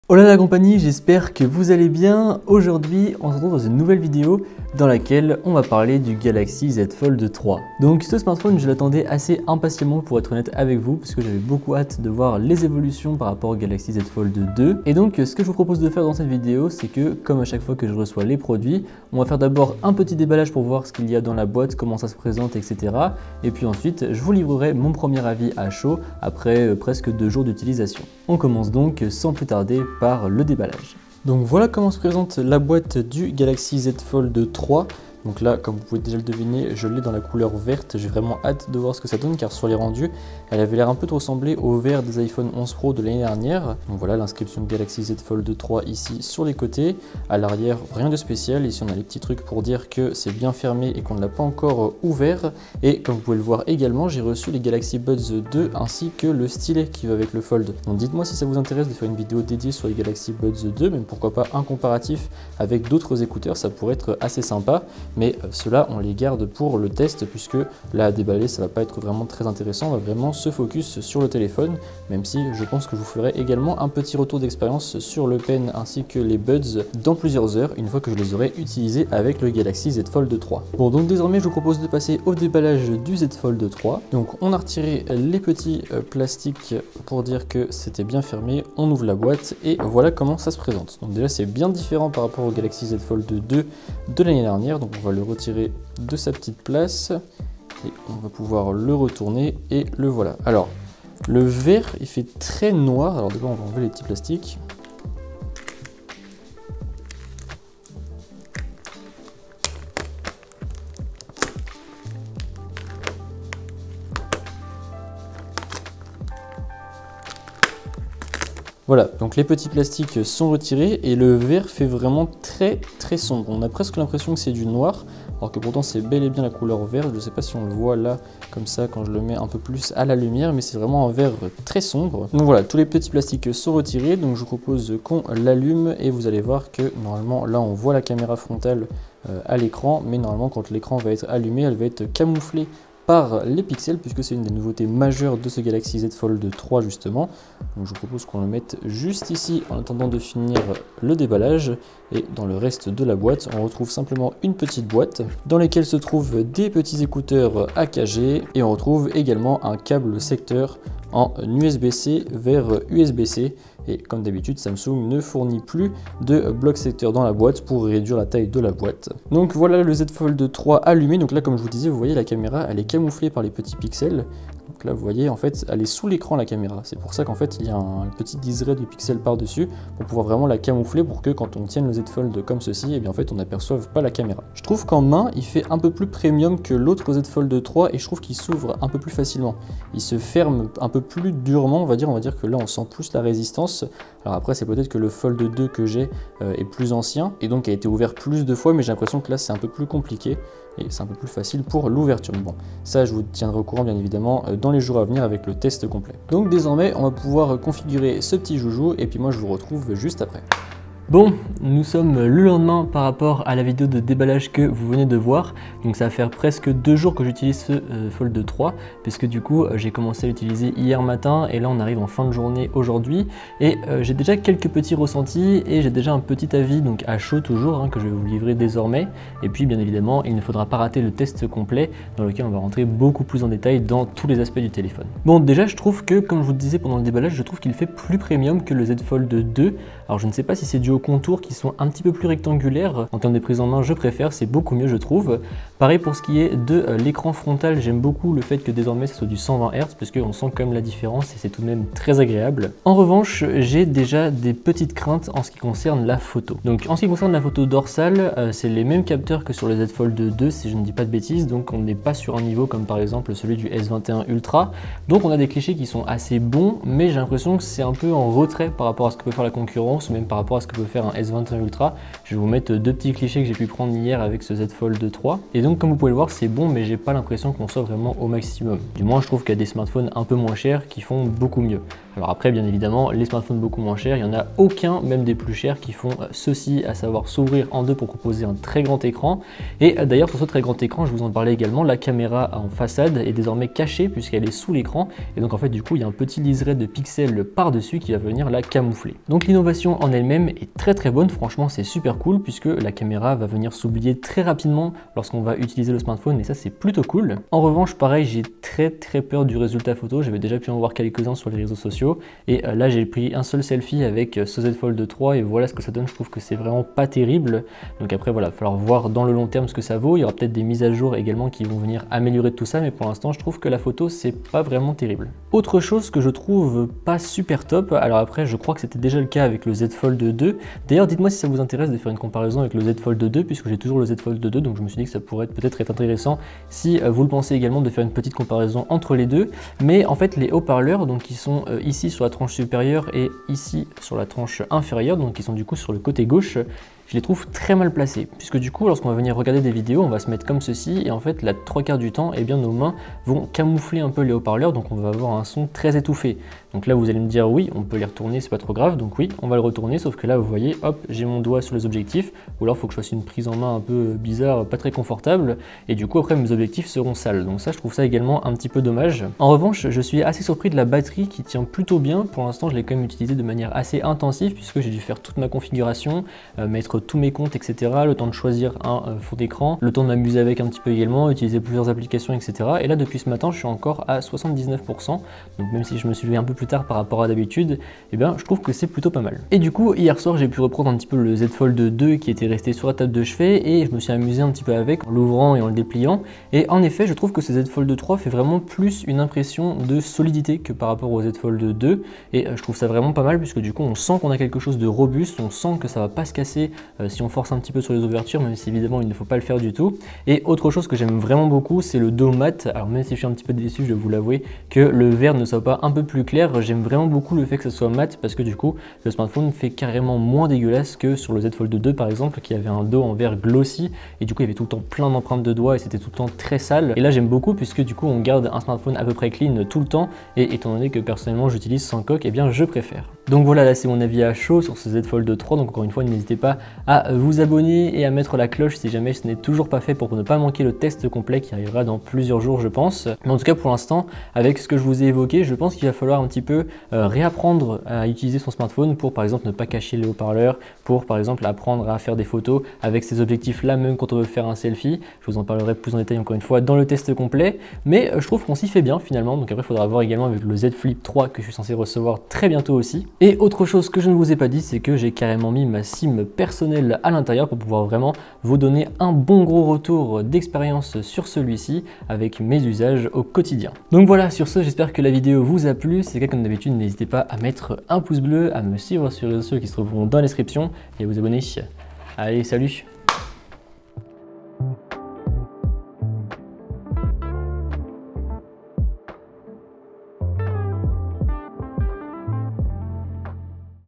J'ai reçu le Galaxy Z Fold 3 ! (Avis à chaud après 2 jours d'utilisation)
Aujourd'hui on déballe le Z fold 3 et je vous livre un premier avis à chaud sur celui-ci.

Le Z Fold 3